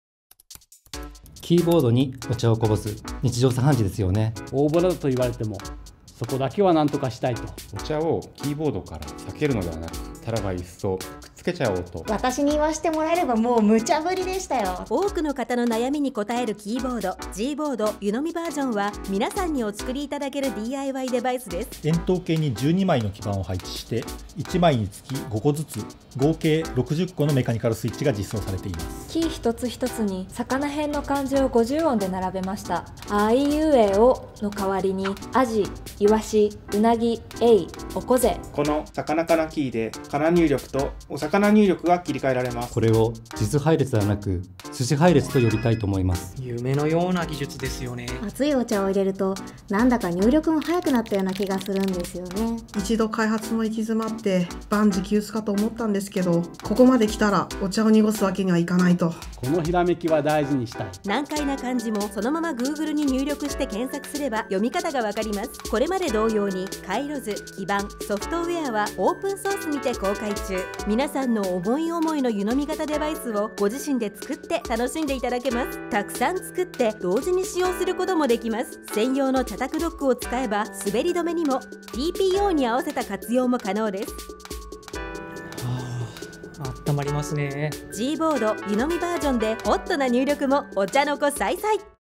Gboard 湯呑みバージョン
Gboard チームからの新しいご提案、Gboard 湯呑みバージョンをご紹介します。
お茶を飲んでいたら、湯呑みを倒してキーボードにお茶をこぼしてしまった。そういうお茶目ながらも苦々しい失敗をさけるために、我々が新しく考案したキーボードです。

詳しくは紹介ページと Google Japan ブログをご覧ください。

この動画は 2020 年 3 月以前に撮影されたものです。

Today, we are proud to announce the new version of Gboard -- the Teacup Version.